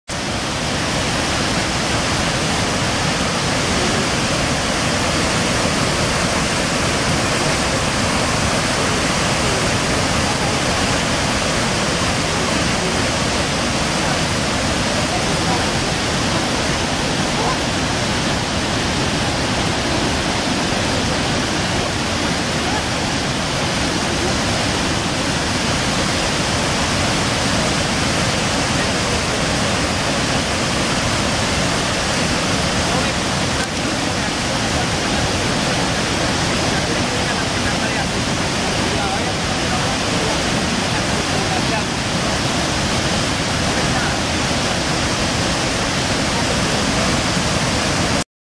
Sapo Falls view from top with Kuravaina + Kursari Tepuy - Canaima National Park
view from the top of the falls to savannah and mountains